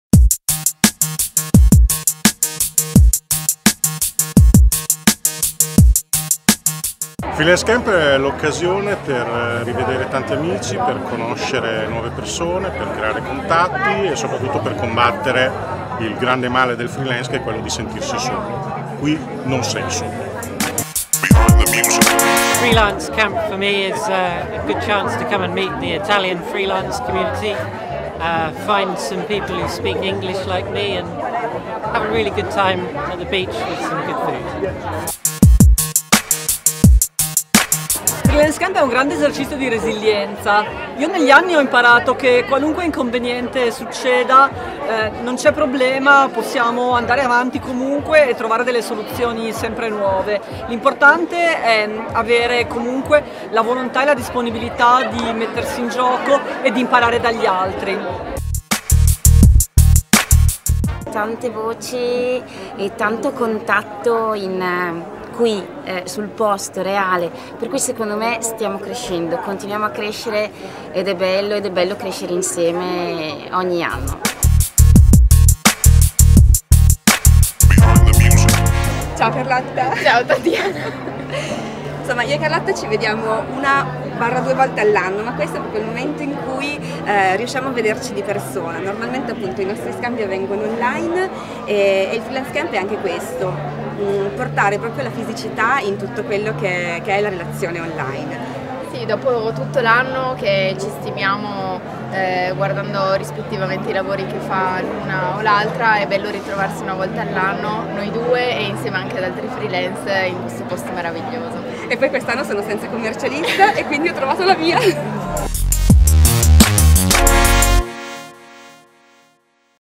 freelancecamp MR18 clip giorno 2
Domenica 13 maggio 2018, secondo giorno del Freelancecamp Marina Romea. Le interviste ai partecipanti e agli speaker.